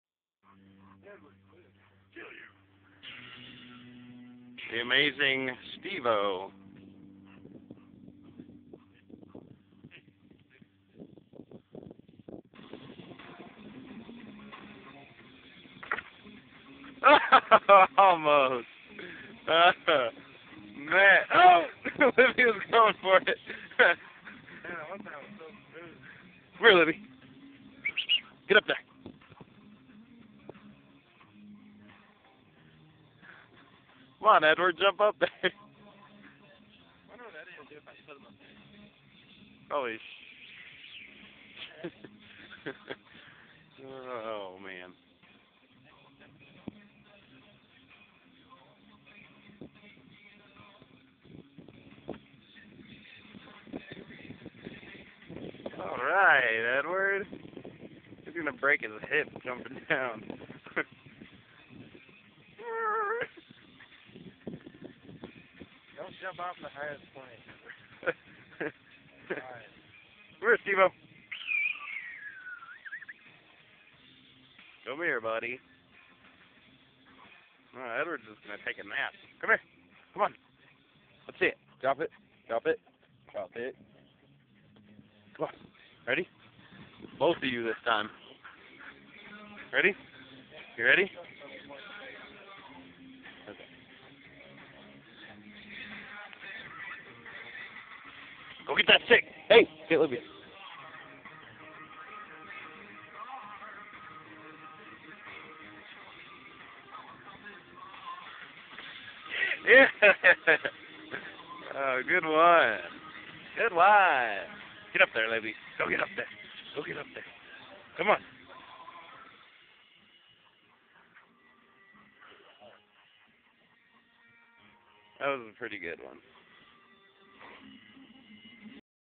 Steve-O and Libbee
Steve-O jumping over a wall onto some hay bales. Libbee being awesome as usual. and Edward the German being weird as usual.